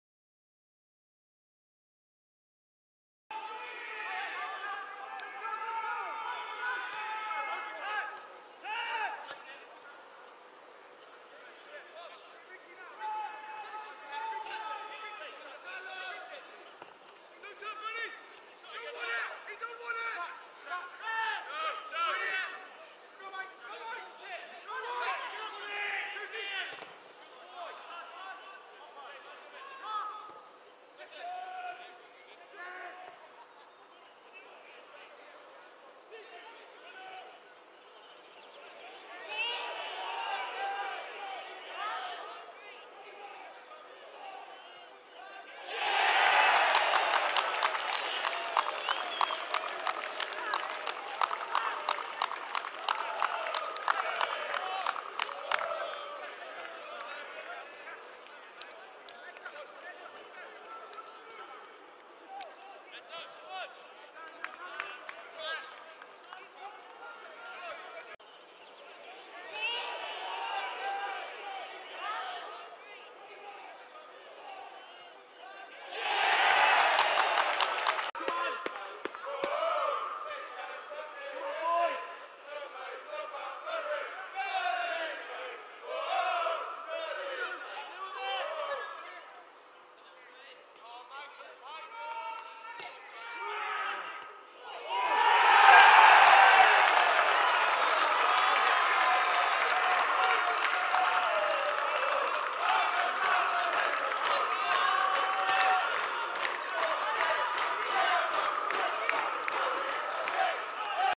Banbury United 2 Bridgwater Town 1 - 1st Mar 2016
The two Banbury goals